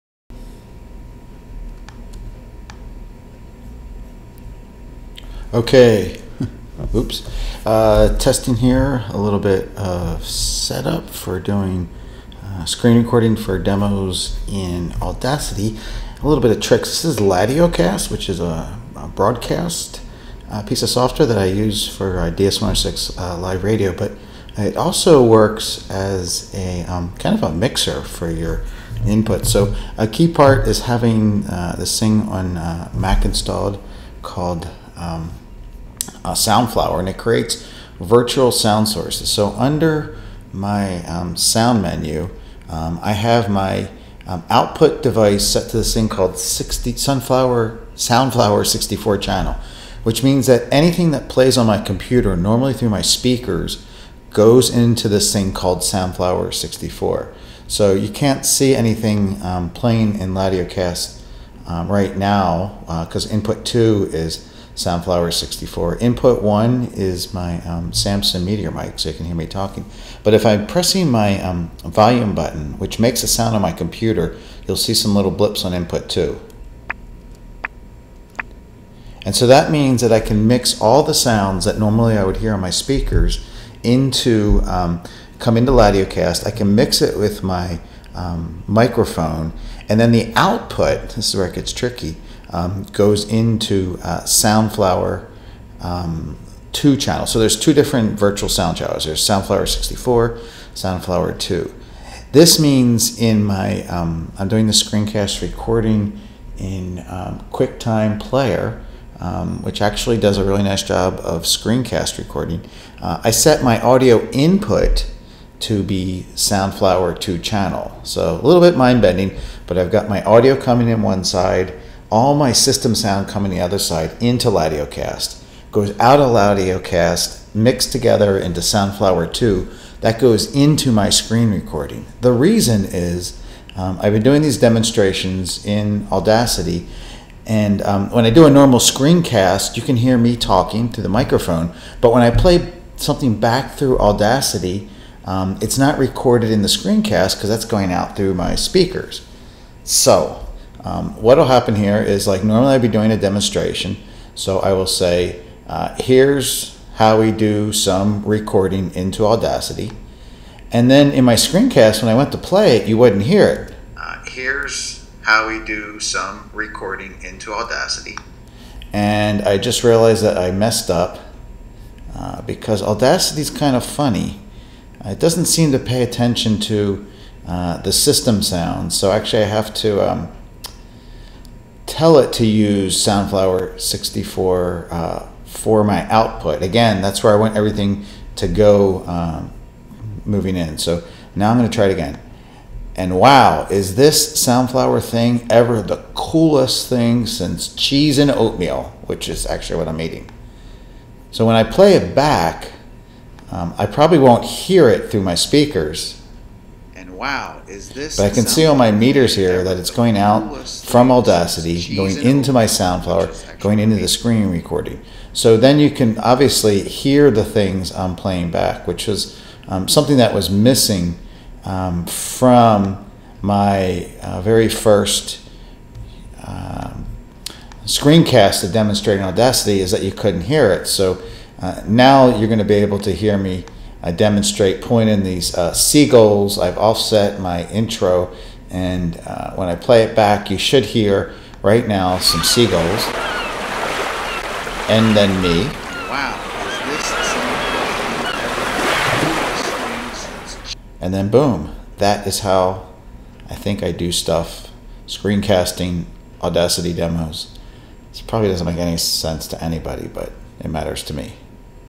Ladiocast Inception Setup For Screen Recording Audacity
Terribly geeky. i've been doing screencasts for using Audacity, and its really tricky to record your voice as explaining AND playing back what you are doing in Audacity. Here is how I do it by using Ladiocast as a mixer